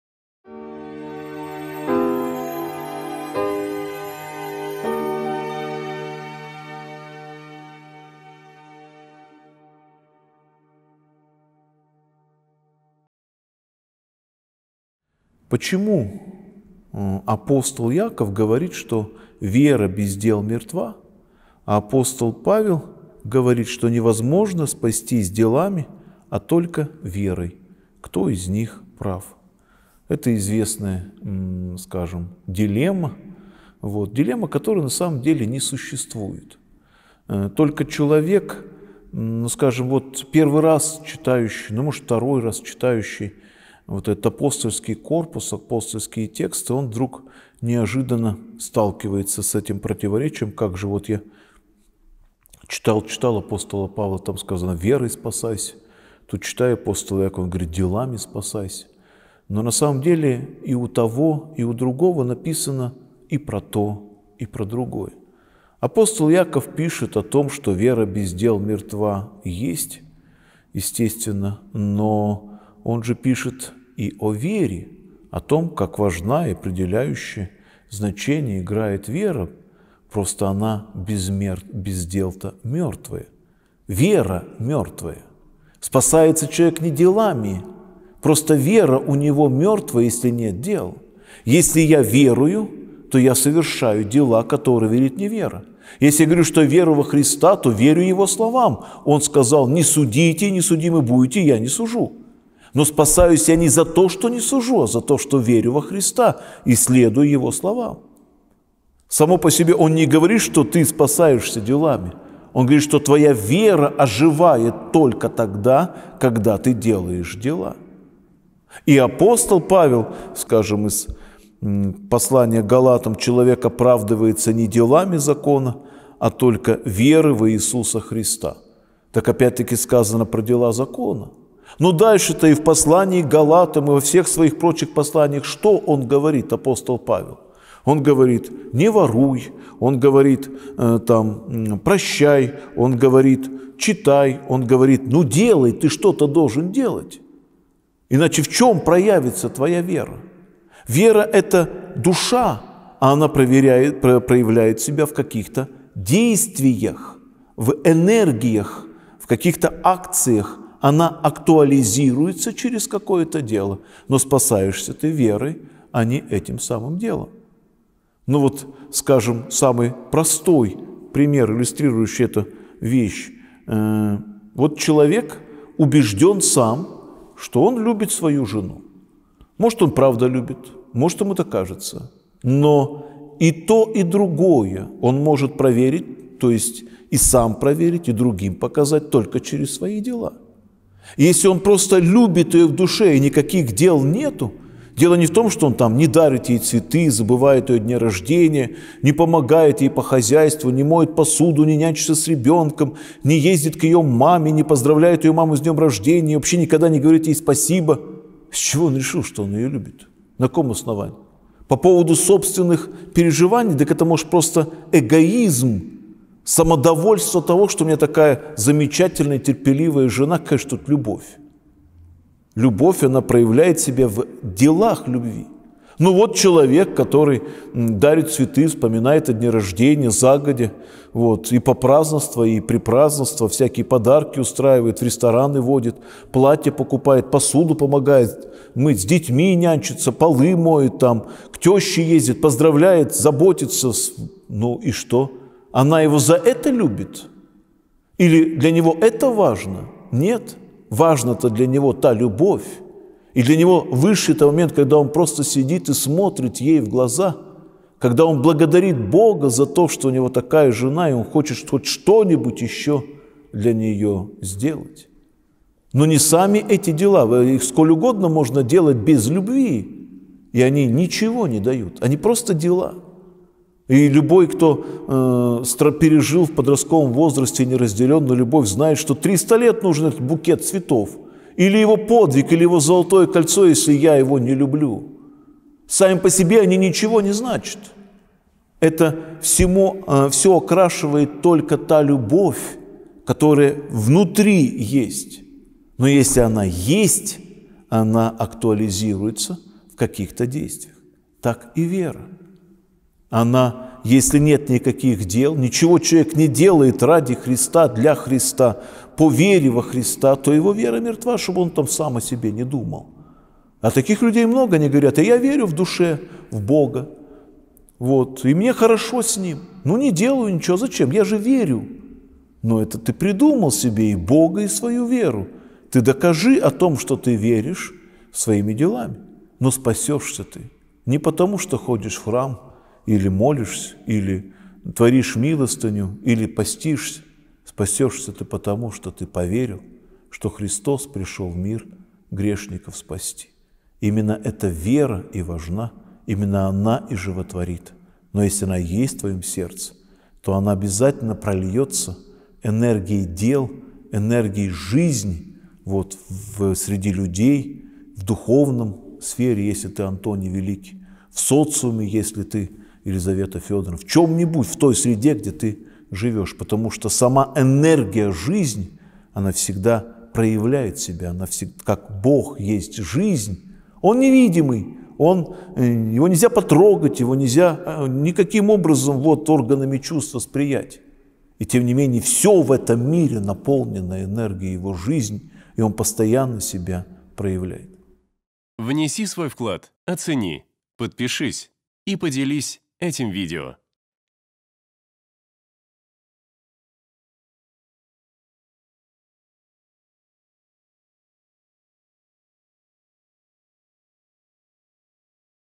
ВЕРА или ДЕЛА ? Священник Константин Корепанов
В сегодняшнем видео священник Константин Корепанов отвечает на вопрос: "Что важнее : вера или дела?"
Касаемся следующих тем:

🔸 Спасение верой.
🔸 Добрые дела.
🔸 Вера в Иисуса Христа.
🔸 Дела закона.
🔸 Любовь в душе.
🔸 Проявление христианской любви.

📜 Читать Послание ап. Иакова, Глава 2, стих 26 (с толкованиями)